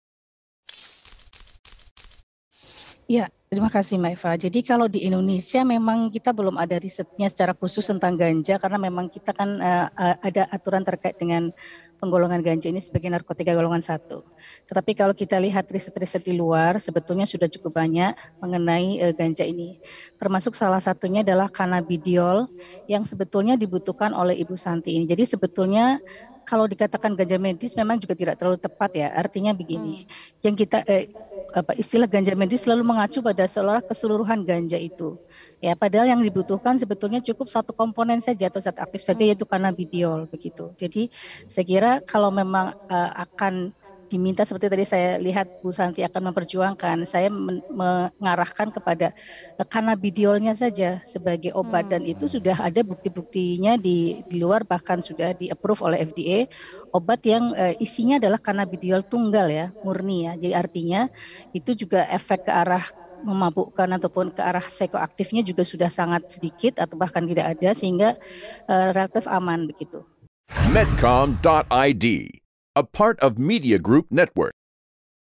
Guru Besar Farmasi UGM: Bukan Ganja yang Dibutuhkan, Tapi Cannabidiol
Zullies Ikawati sebagai guru besar fakultas farmasi UGM menjelaskan bahwa memang Indonesia tidak mempunyai riset-riset mengenai ganja karena aturan hukum yang berlaku. Tetapi berdasarkan riset-riset yang ada dari luar negeri, bahwa yang dibutuhkan oleh kasus anak Bu Santi bukanlah ganja. Tetapi zat aktif yang ada dalam ganja, yaitu Cannabidiol.
Zullies juga mengarahkan kepada Bu Santi untuk menggunakan obat cannabidiol tunggal untuk meredakan kejang-kejang pada anaknya dan relatif lebih aman untuk digunakan.